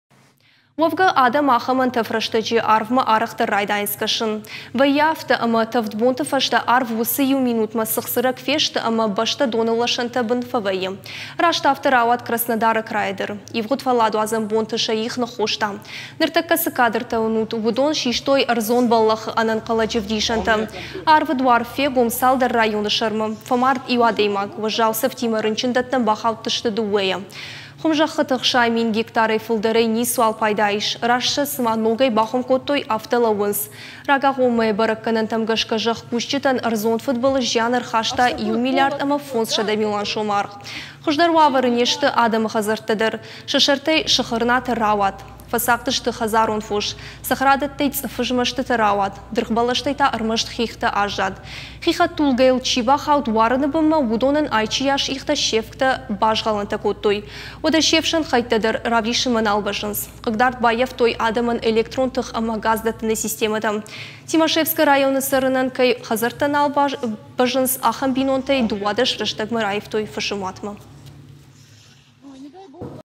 Æрдзон фыдæх
Уæвгæ адæм ахæм æнтæф рæстæджы арвмæ арæхдæр райдайынц кæсын. Вæййы афтæ, æмæ тæвд бонты фæстæ арв уыцы-иу минутмæ цыхцырæг фесты æмæ бæстæ доны лæсæнты бын фæвæййы.

Раст афтæ рауад Краснодары крайы дæр. Ивгъуыд фæлладуадзæн бонты сæ их ныххоста. Ныртæккæ цы кадртæ уынут, уыдон систой æрдзон бæллæхы æнæнхъæлæджы `вдисæнтæ. Арвы дуар фегом цалдæр районы сæрмæ. Фæмард иу адæймаг, уæззау цæфтимæ рынчындæттæм бахаудысты дыууæйæ. Хуымзæххыты 60 мин гектарæй фылдæрæй ницыуал пайда ис. Раст сæ цыма ногæй бахуым кодтой, афтæмæй лæууынц. Рагагъоммæйы бæрæггæнæнтæм гæсгæ, зæхкусджытæн æрдзон фыдбылыз зиан æрхаста иу миллиард æмæ 500 милуан сомы аргъ. Хуыздæр уавæры не сты адæмы хæдзæрттæ дæр. Сæ сæртæй сыхырнатæ рауад. Фæцагъды сты хæдзарон фос, цæхæрадæттæй цъыфы змæстытæ рауад, дыргъбæлæстæй та æрмæст хихтæ аззад. Хи хæдтулгæйыл чи бахауд уарыны бынмæ, уыдонæн айчы йас ихтæ се `вгтæ базгъæлæнтæ кодтой. Уæдæ се `фсæн хæйттæ дæр равдисынмæ нал бæззынц. Хъыгдард баййæфтой адæмæн электрон тых æмæ газ дæттыны системæтæ. Тимашевскы районы цæрынæн кæй хæдзæрттæ нал бæззынц, ахæм бинонтæй 12 рæстæгмæ раивтой фысымуатмæ.